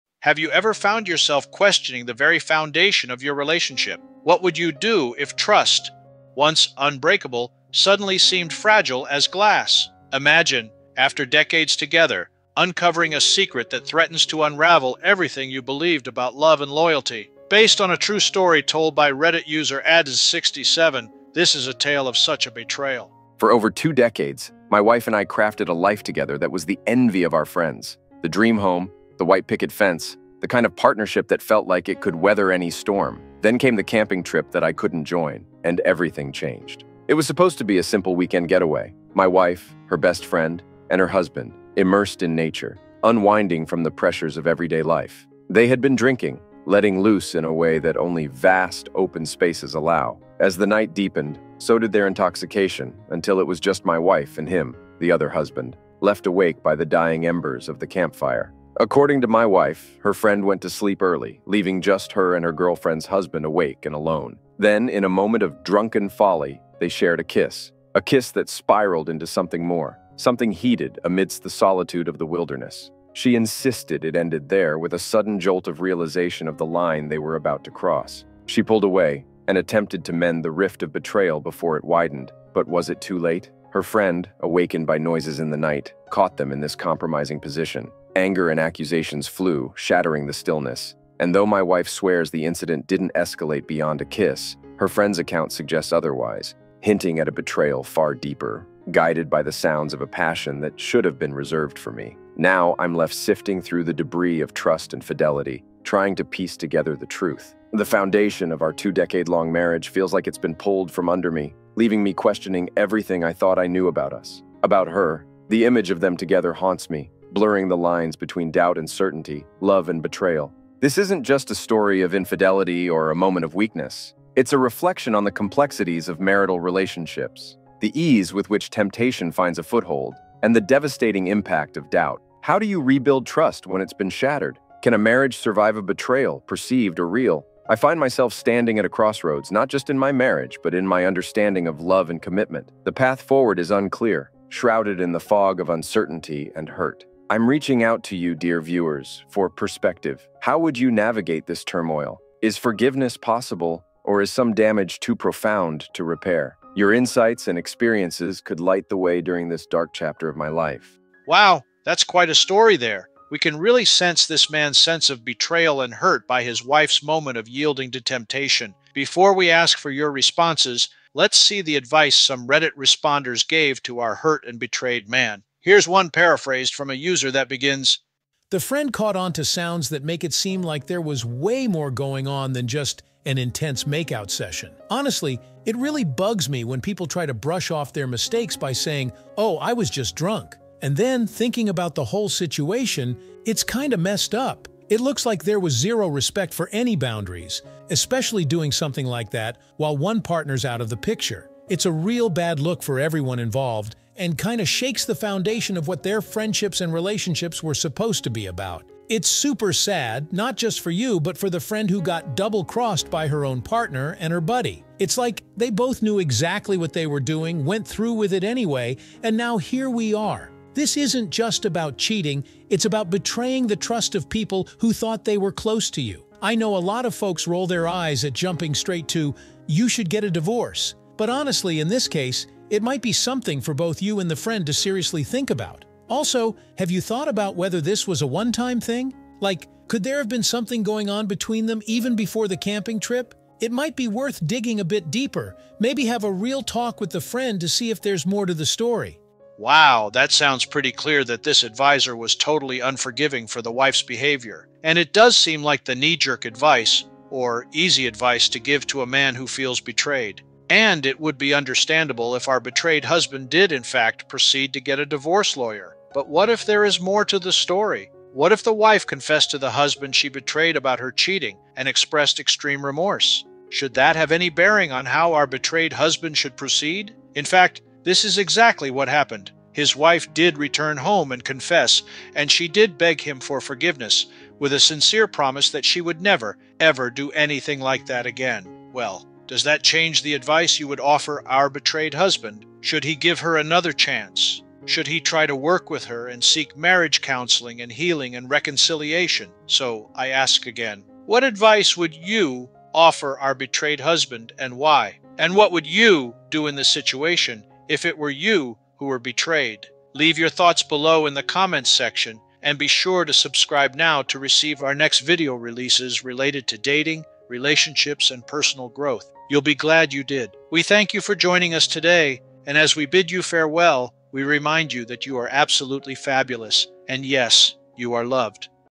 Cheating Wife Shock: What Would YOU Do After This Betrayal?
In a tale as old as time, but with a twist that cuts deep into the heart of modern marriage, "Reddit Wife Cheating Shock: What Would YOU Do After This Betrayal?" unveils a story that has the internet ablaze. When trust, the cornerstone of a relationship, crumbles into doubt, how does one navigate the debris of betrayal? This video delves into a real-life saga shared by a Reddit user, adzzz67, whose world turned upside down in the wake of unexpected infidelity.

Imagine dedicating over two decades of your life to building a dream with someone, only to find that dream threatened by a single night of indiscretion. This story isn't just about the act of cheating; it's a profound exploration of love, trust, and the complex tapestry of human emotions that are entwined within the sanctity of marriage. From the serene setting of a camping trip gone wrong to the stormy aftermath of revealed secrets, we take you on a journey through the eyes of those embroiled in the heartache.

But this video goes beyond just recounting a tale of marital betrayal. It poses a pressing question to its viewers: What would YOU do if placed in a similar situation? The comments section below is a space for you to share your thoughts, experiences, and advice on dealing with the aftermath of infidelity. Whether it's seeking reconciliation or finding the strength to start anew, your input is invaluable to those facing their darkest moments.

In an era where the sanctity of relationships is constantly tested, "Reddit Wife Cheating Shock" serves as a poignant reminder of the fragility of trust and the resilience required to mend a broken heart. Join us as we unpack the layers of this compelling story, offering insights, empathy, and a shared space for healing.

"Reddit Wife Cheating Shock: What Would YOU Do After This Betrayal?" is more than just a video; it's a journey into the depths of human relationships, inviting you to ponder, participate, and perhaps find peace in the shared experiences of others. Watch now to dive into the discussion and discover how you would navigate the treacherous waters of betrayal and trust.